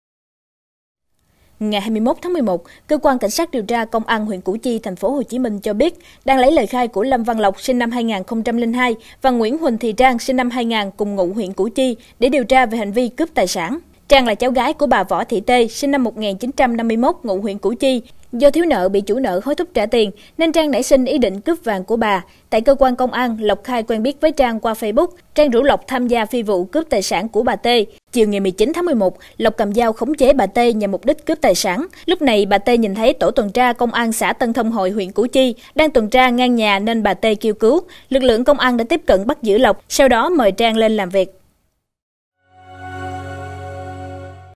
Cháu gái rủ trai về nhà khống chế bà, cướp vàng
Ngày 21-11, Cơ quan CSĐT Công an huyện Củ Chi (TP HCM) cho biết đang lấy lời khai của Lâm Văn Lộc (SN 2002) và Nguyễn Huỳnh Thùy Trang (SN 2000, cùng ngụ huyện Củ Chi) để điều tra về hành vi "Cướp tài sản".